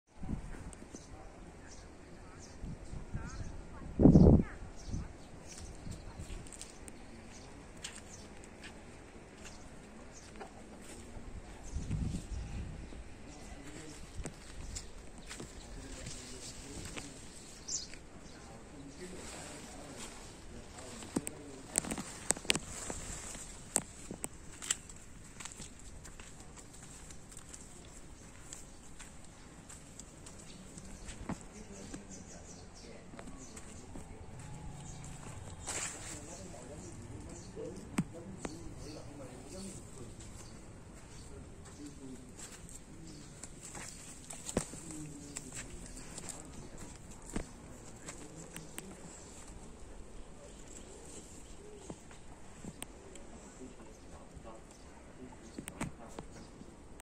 Vườn Hồng Cổ Sapa trồng bịch
Vườn hồng cổ sapa thân tơ trồng phát triển rất nhanh. mới 4 tháng mà tán đã tròn đều rất đẹp